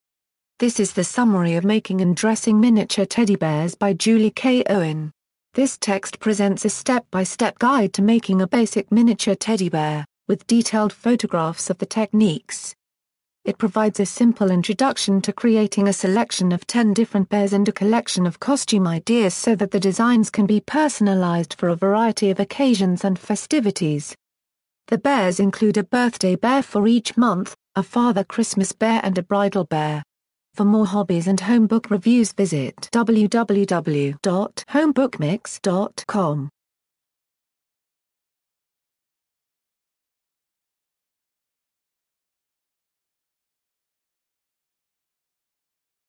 Home Book Summary: Making and Dressing Miniature Teddy Bears by Julie K. Owen
This is the review of Making and Dressing Miniature Teddy Bears by Julie K. Owen.